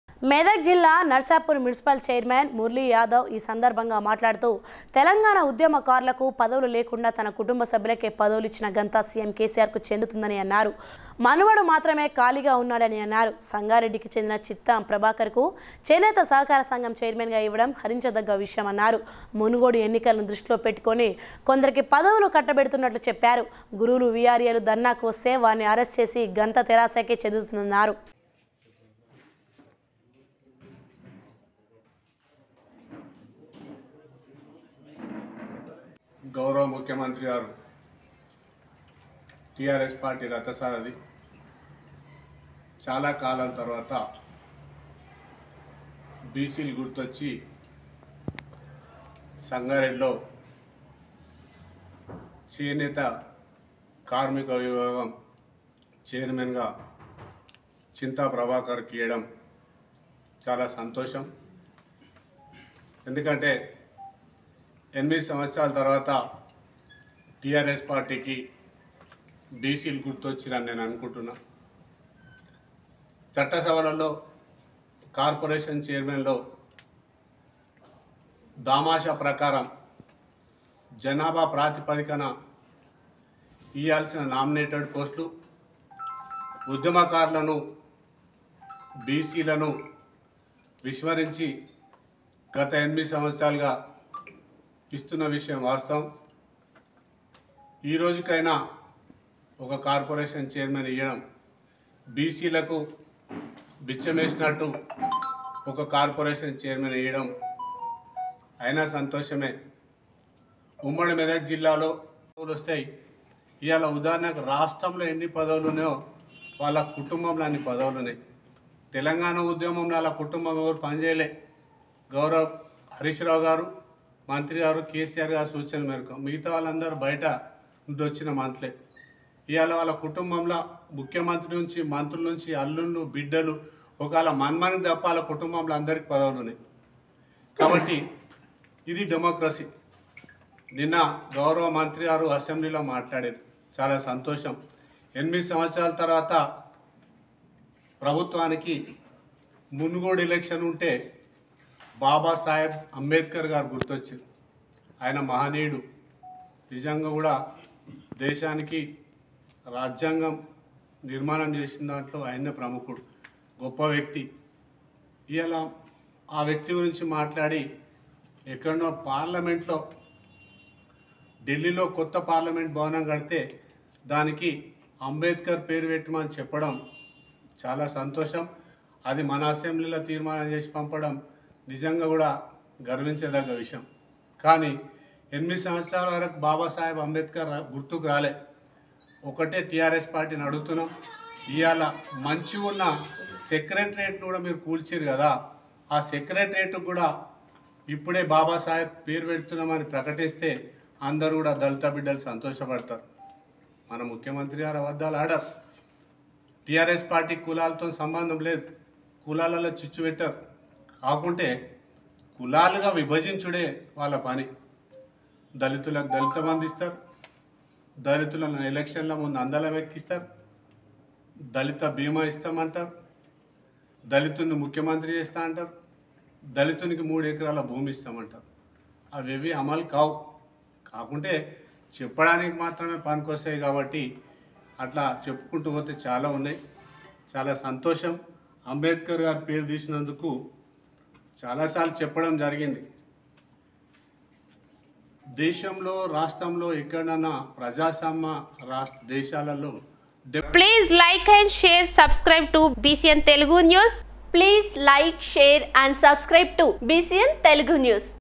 కెసిఆర్ కుటుంబ సభ్యులకే పదవులు : నర్సాపూర్ మున్సిపల్ చైర్మన్ మురళి || Medak
మెదక్ జిల్లా ,నర్సాపూర్ మున్సిపల్ చైర్మన్ మురళి యాదవ్ ఈ సందగా మాట్లాడుతూ ,తెలంగాణ ఉద్యమకారులకు పదవులు లేకుండా తన కుటుంబ సభ్యులకే పదవులు ఇచ్చిన ఘనత సీఎం కేసిఆర్ కు చెందుతుందని అన్నారు మనుమడు మాత్రమే ఖాళీగా ఉన్నాడని అన్నారు. సంగారెడ్డి కి చెందిన చింత ప్రభాకర్ కు చేనేత సహకార సంఘం చైర్మన్ గా ఇవ్వడం హర్షించదగ్గ విషయం అన్నారు. మునుగోడు ఎన్నికల ను దృష్టిలో పెట్టుకొని కొందరికి పదవులు కట్టబెడుతున్నట్లు చెప్పారు .గురువులు ,వీఆర్ఏలు ధర్నాకు వస్తే వారిని అరెస్టు చేసిన ఘనత తెరాసకే చేరుతుందన్నారు
         Medak District, Narsapur Municipal Chairman Murali Yadav said on this occasion that CM KCR has the honor of giving posts to his family members without Telangana activists and said that only his grandson is vacant. Chinta Prabhakar from Sangareddy was given the chairmanship of handloom co-operative society, he said. He said that they are giving posts to some people keeping in mind the previous elections. If teachers and VRAs come to dharna, the credit of arresting them will go to Terasa.